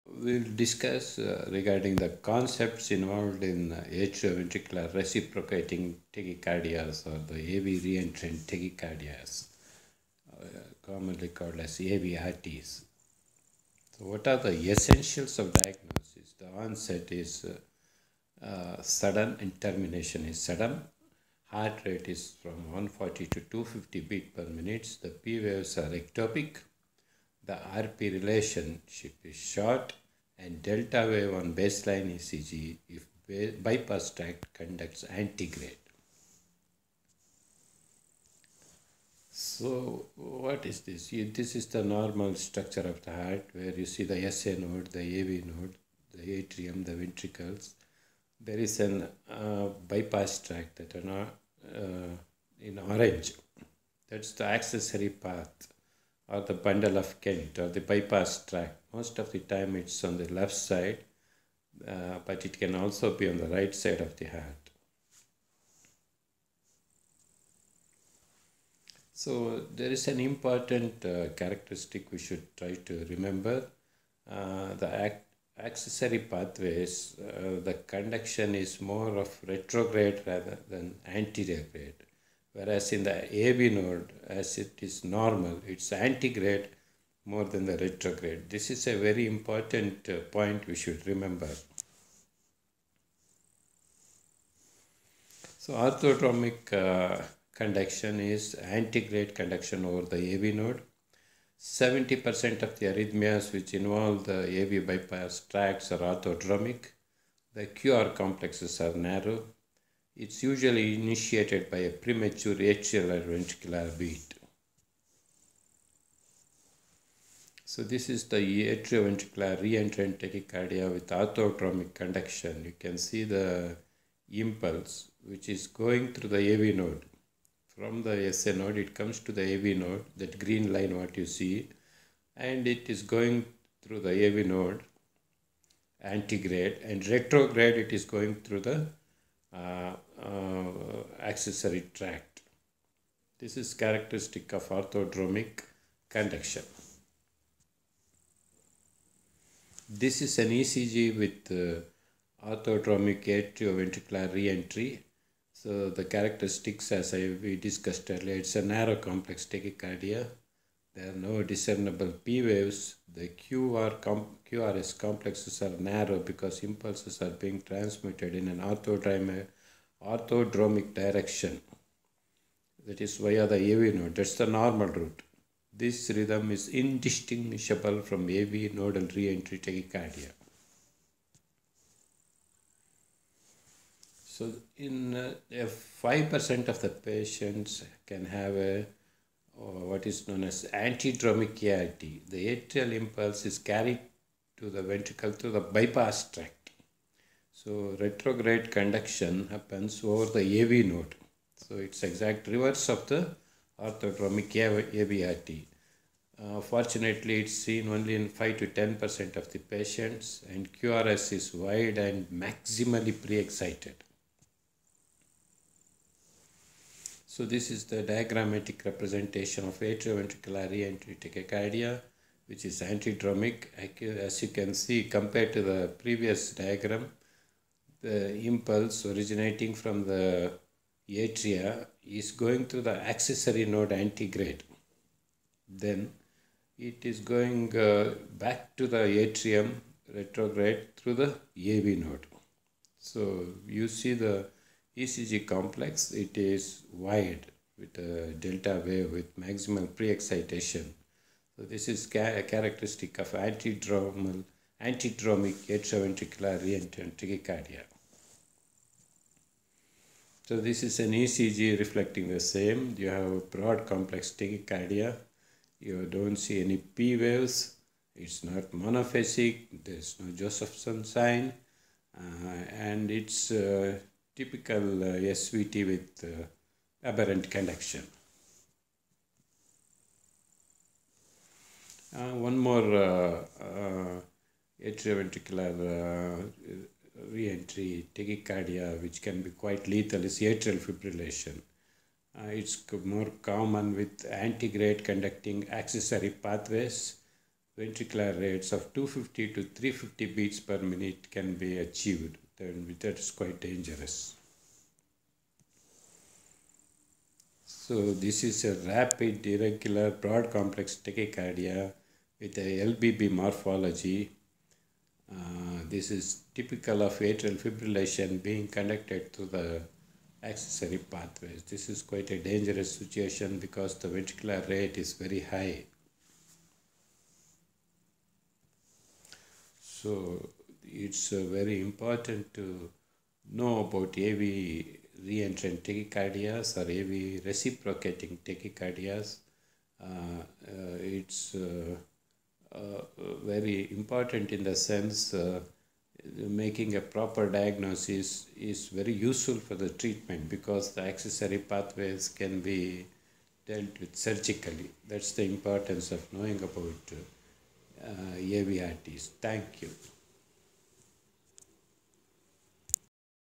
Orthodromic AVRT
Core concepts in understanding AVRT
By Dr.T Anil Kumar
Prof & HOD
Dept of Medicine ,MSRMC